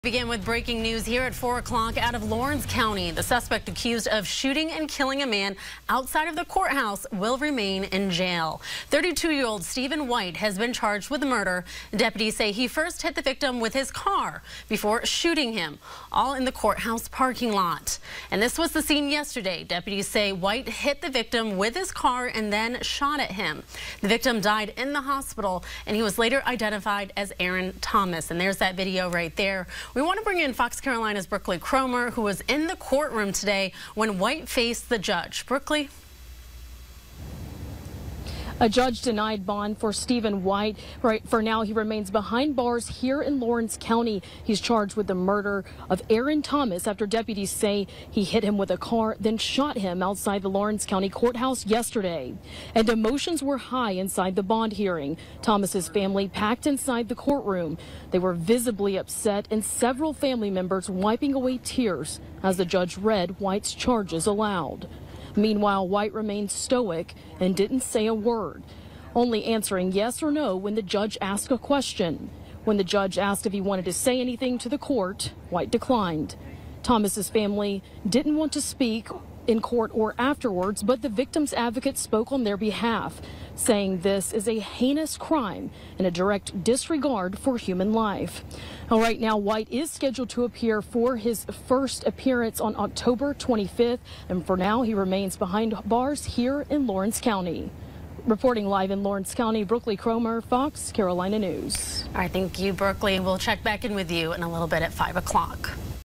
Suspect in Laurens County Courthouse shooting denied bond
Stephen White has been denied bond by a Laurens County judge.  White is charged with murder following a shooting outside the Laurens Co. Courthouse Monday